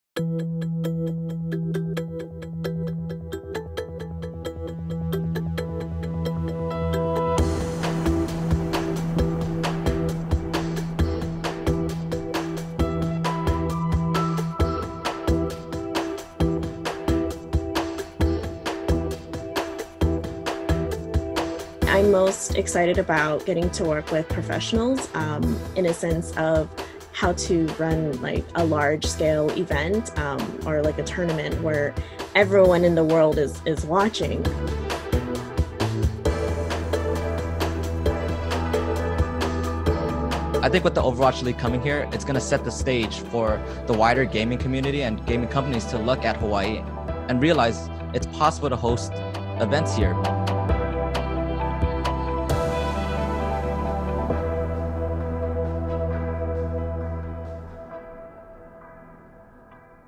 Global elite Overwatch League™ teams to compete at UH
The University of Hawaiʻi at Mānoa will host one of the fastest growing leagues in all of international esports. Elite teams from the Overwatch League (OWL) will participate in a series of four tournaments at UH Mānoa May–August 2021.

Read more about the University of Hawaiʻi: uhnews.org